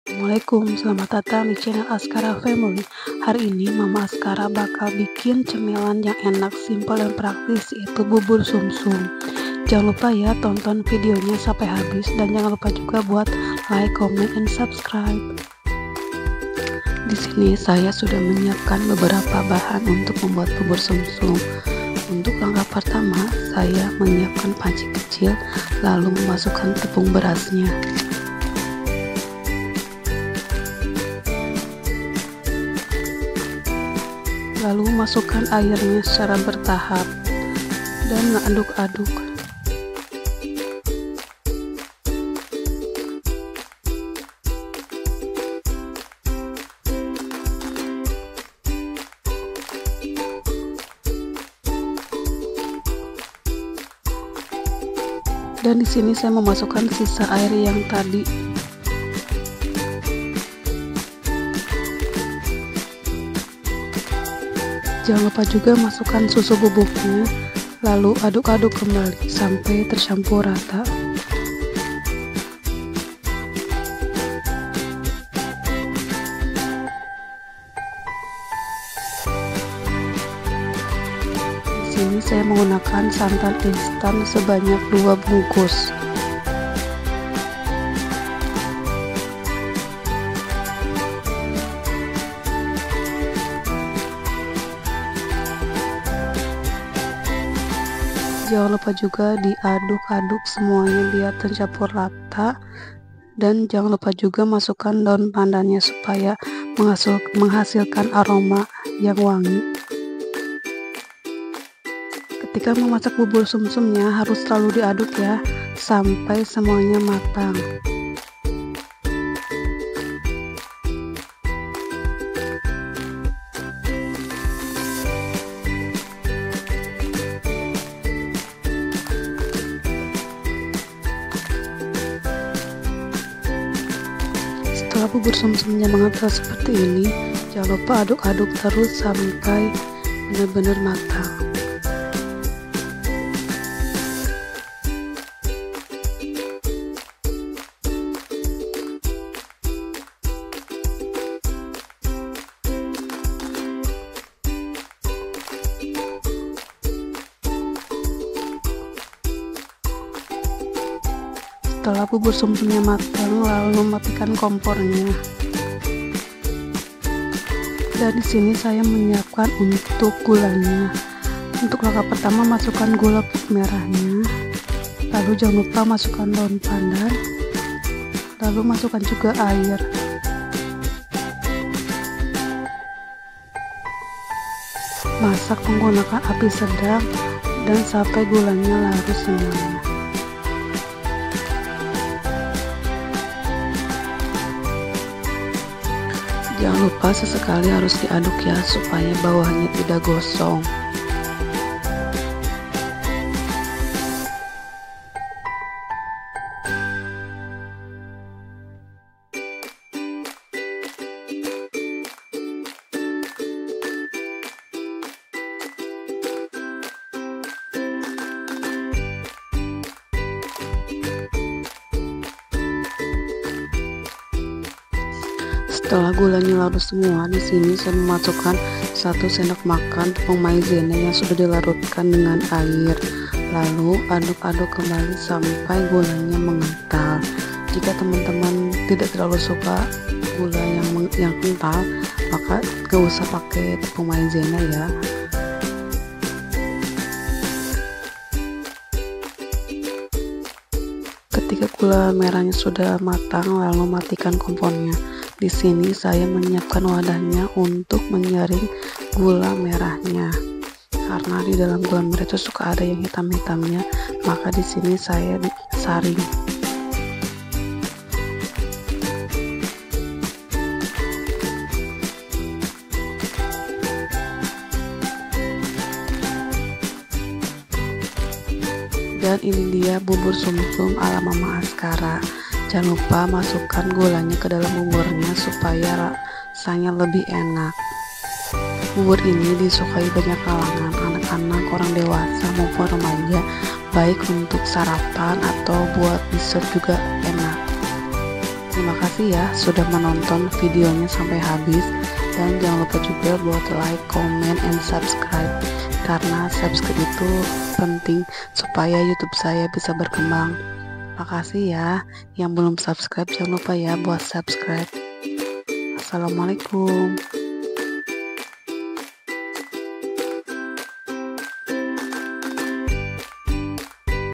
RESEP BUBUR SUMSUM!! ENAK DAN LEMBUT...
Assalamualaikum hari ini mama askara mau bikin cemilan simple lagi yaitu BUBUR SUMSUM yang Enak dan Lembut ..

Resep:
100gr Tepung beras
700ml air
1 lembar daun pandan
1 SASET susu bubuk
2bks santan instan
1/2 sdt garam

Bahan :
Gula merah /aren 200gr
1sdm maizena sudah di larutkan dgn 1sdm air(opsional)
300ml air